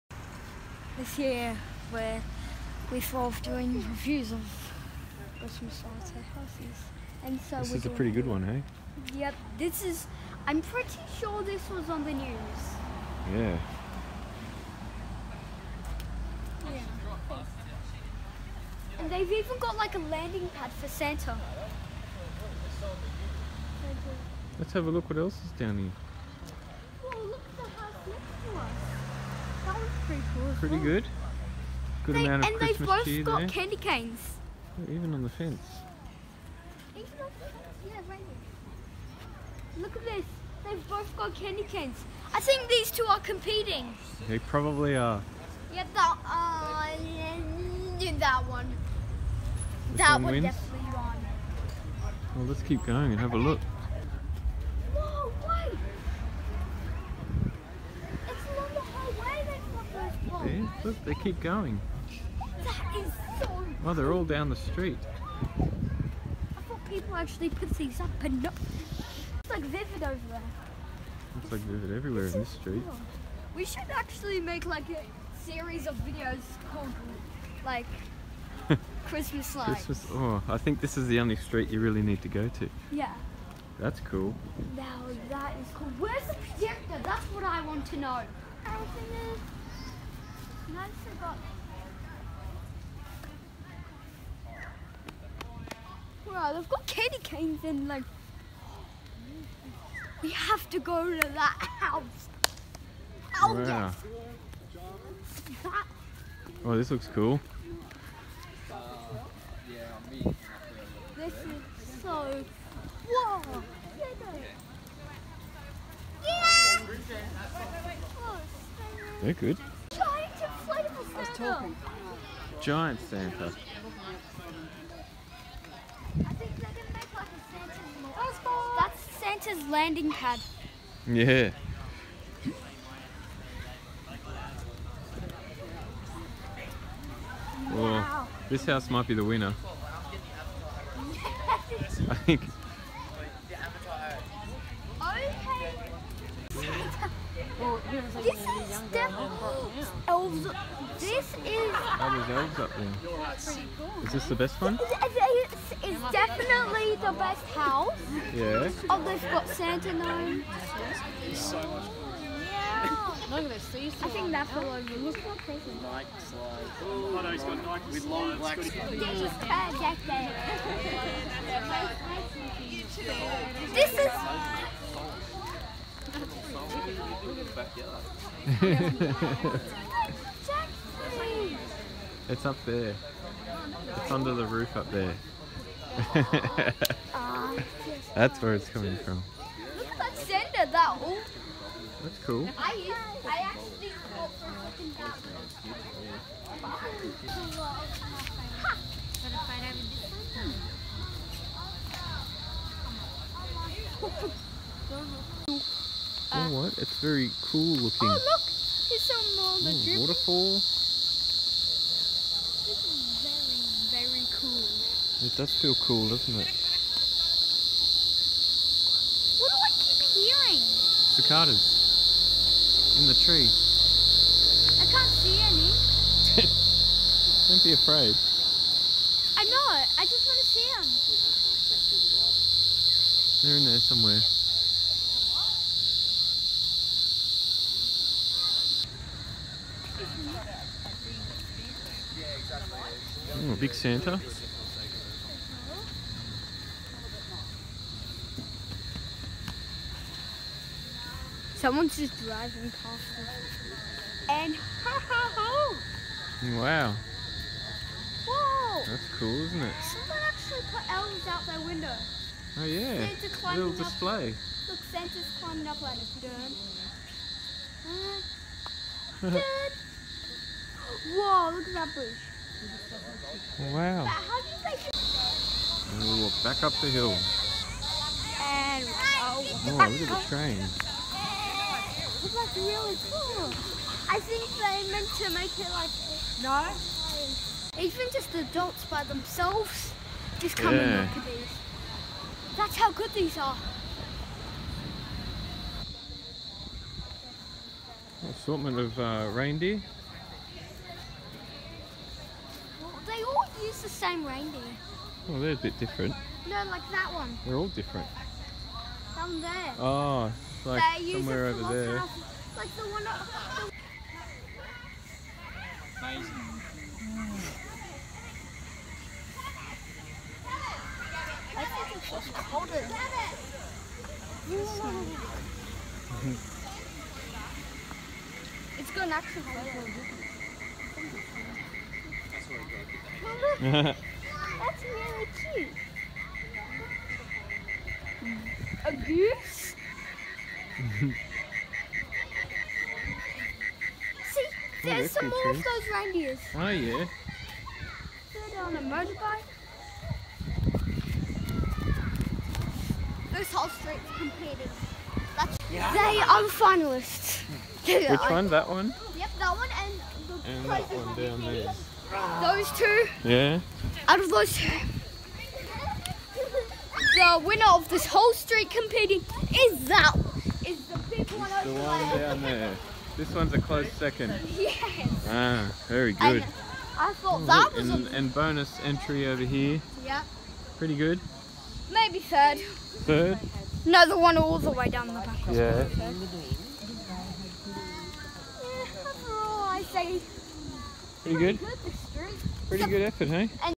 Jack and the Christmas Lights 2017
Jack checks out some amazing Christmas lights in Caringbah.